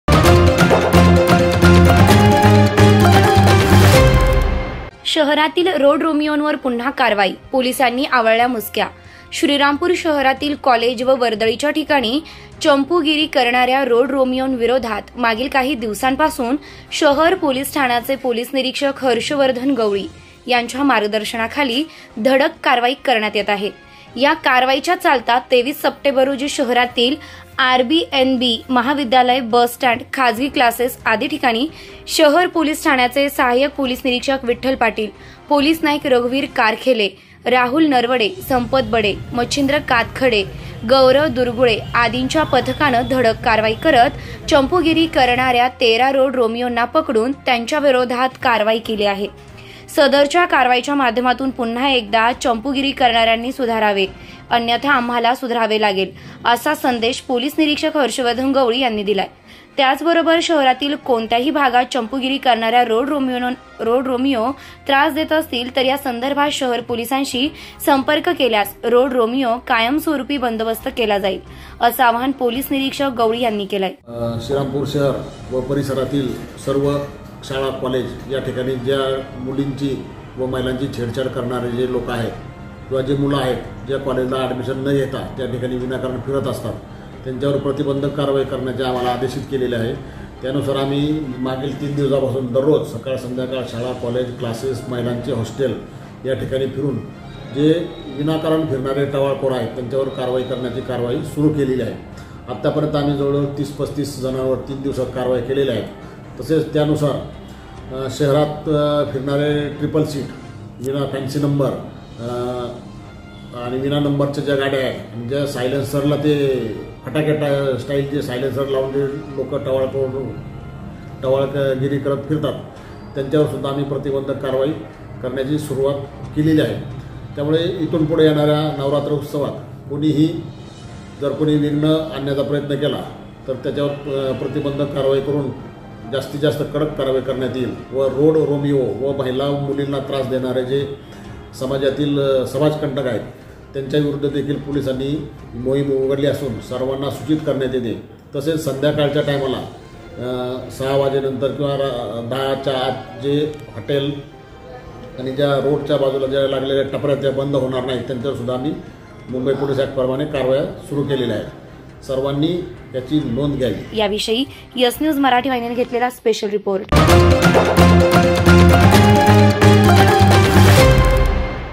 शहरातील रोड रोमिओंवर पुन्हा कारवाई, पोलिसांनी आवळल्या मुसक्या.|SNEWS MARATHI
एस न्युज मराठी हे काम नाही… जबाबदारी आहे आमची.